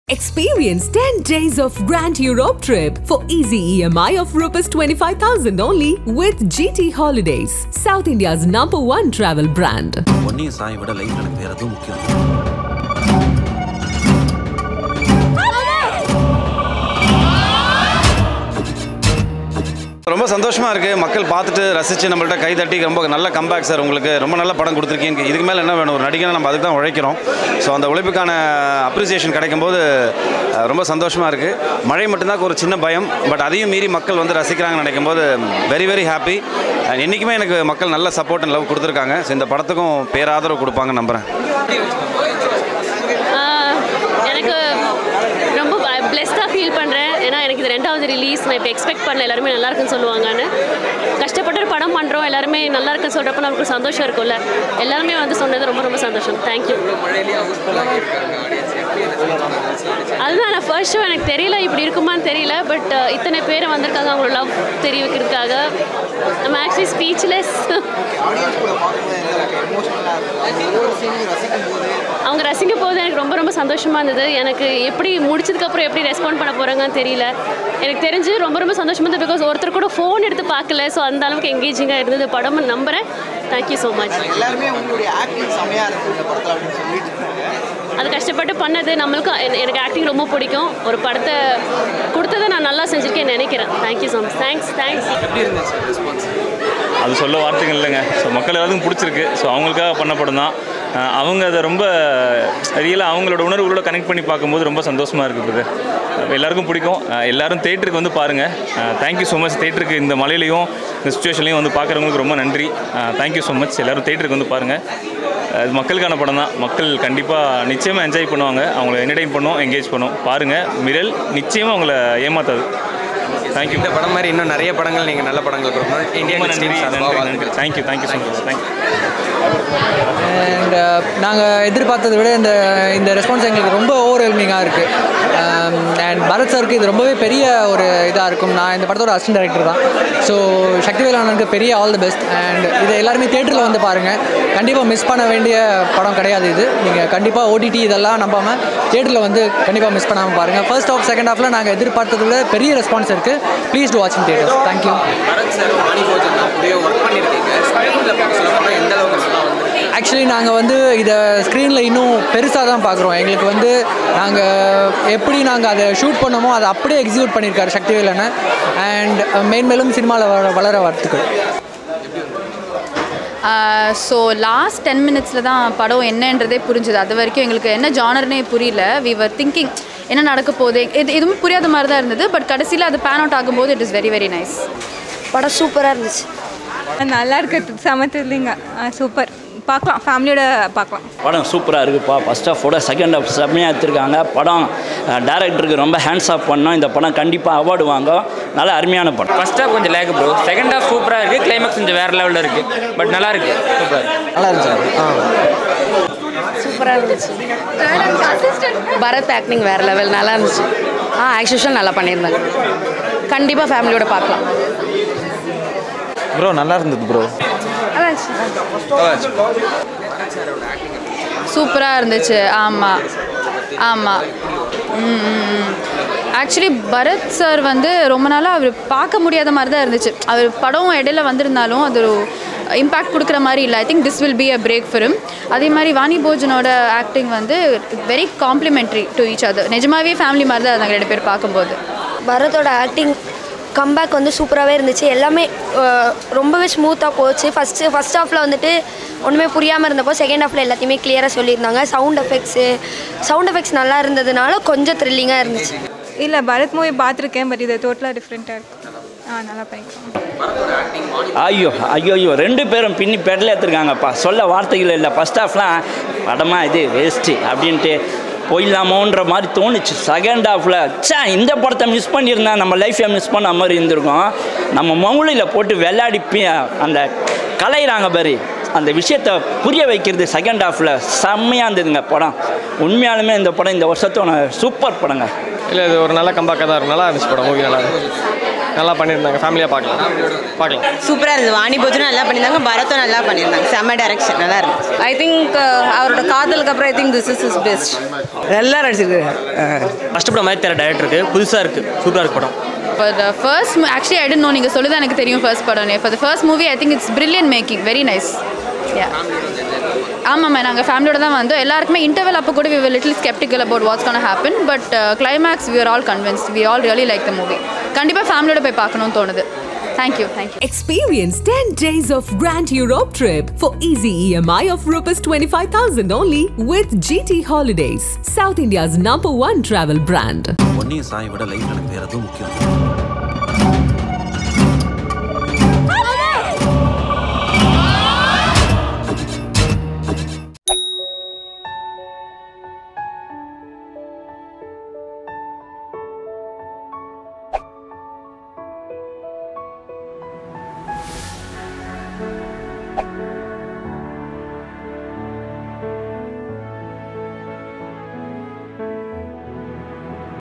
Miral Public Review | Bharath, Vani Bhojan | M Sakthivel | Miral Public Opinion
Don't miss our public review of Miral , directed by M Sakthivel! Hear what our audience has to say about the film and get their reviews and thoughts on the film.

GT Holidays - South India's No.1 Travel Brand.

Are you a Passionate Short Filmmaker here's a Chance for you to showcase your Talent